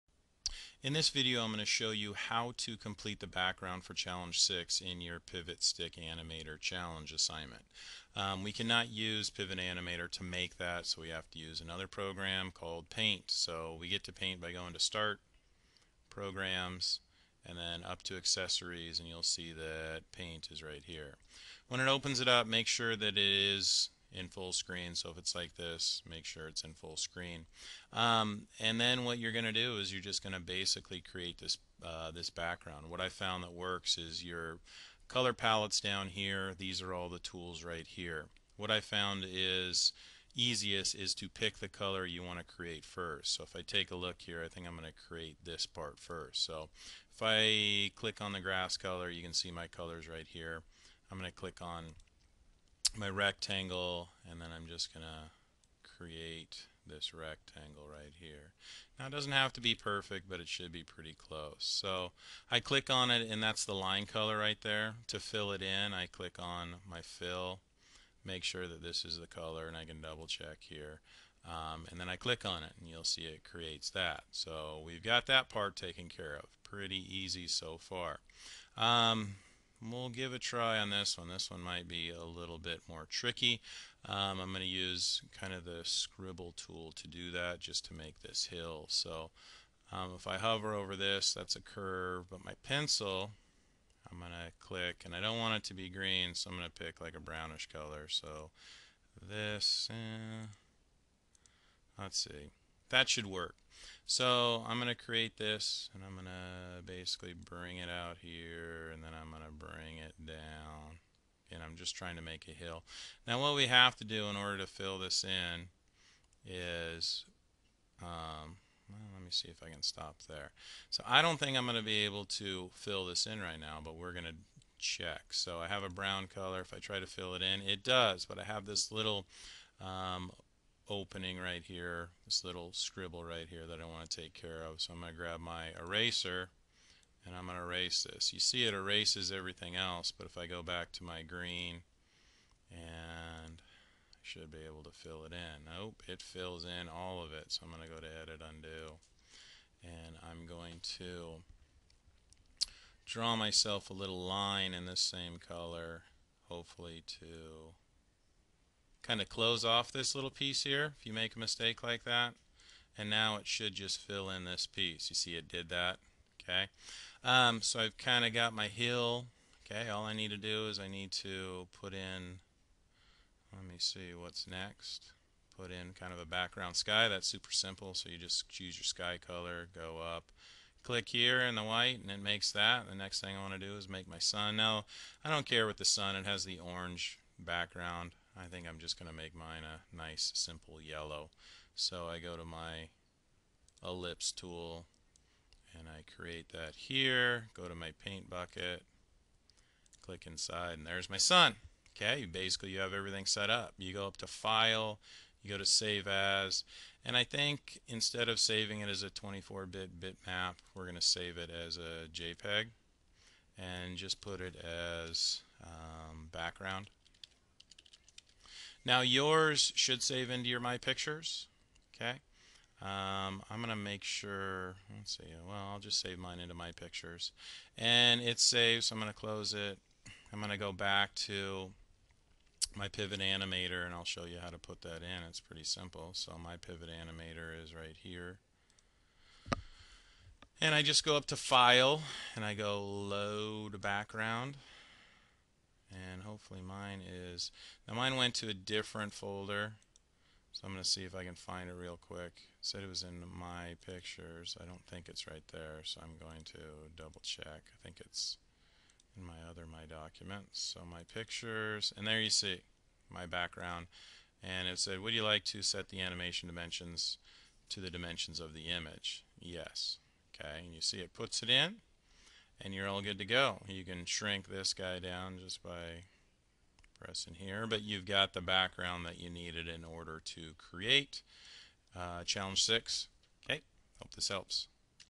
How to create and import in a background using Pivot Animator
This is a short tutorial on how to create a background in paint, then import it into your Pivot animation